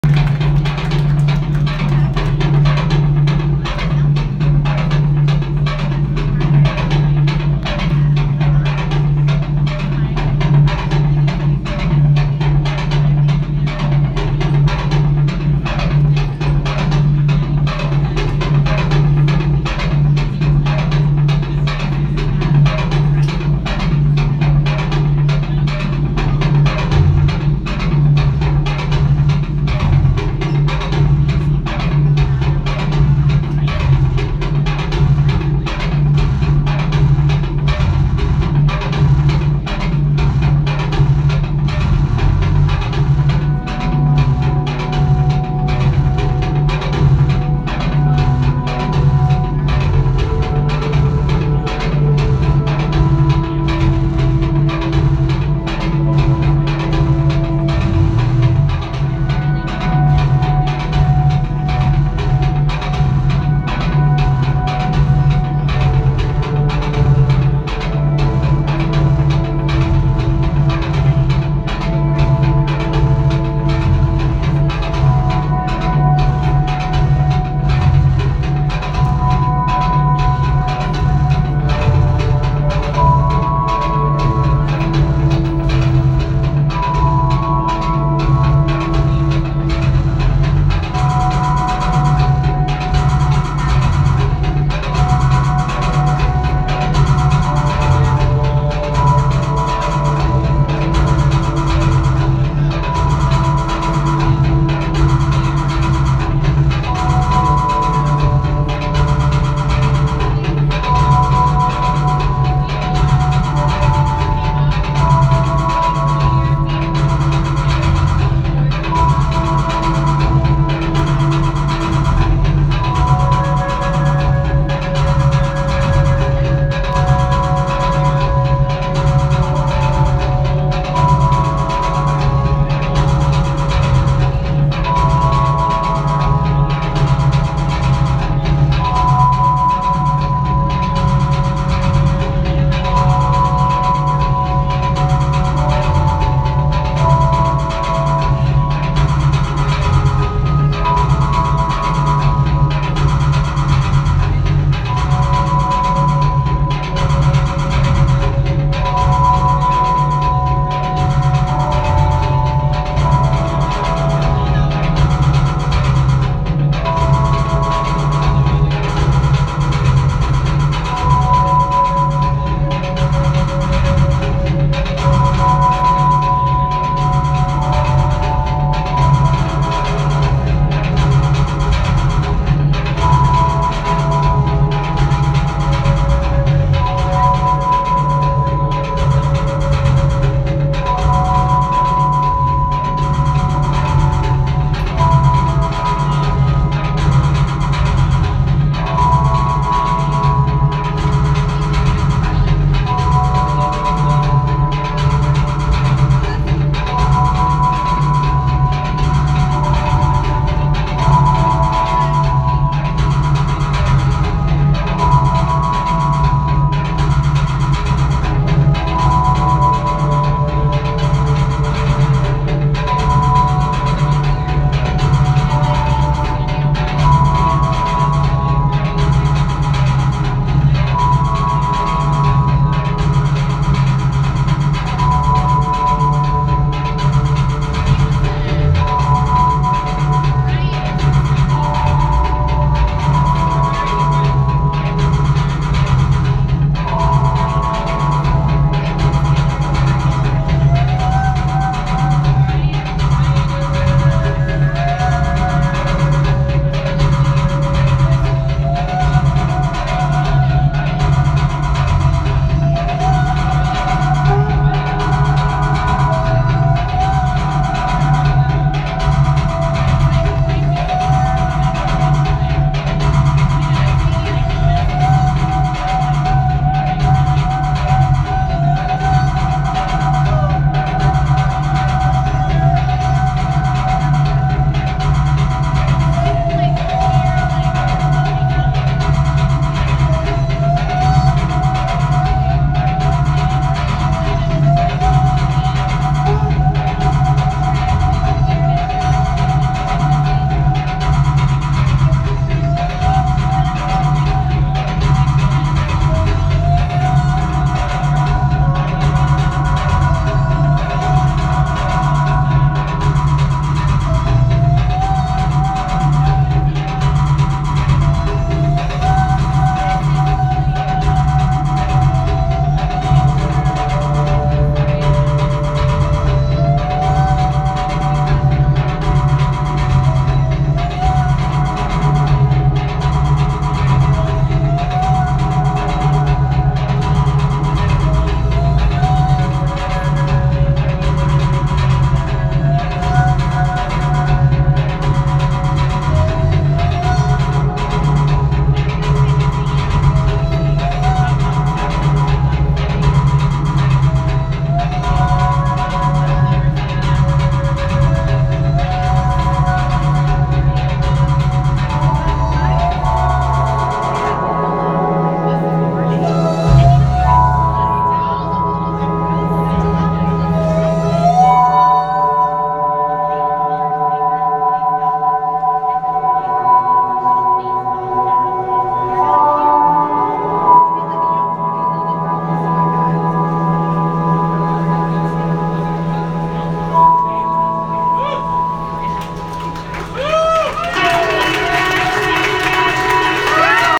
Seefeel_Live at Johnny Brenda's_11-15-19_Song 06
Seefeel - Live at Johnny Brenda's in Philadelphia 11-15-19 - Last Show From 1st Ever North American Tour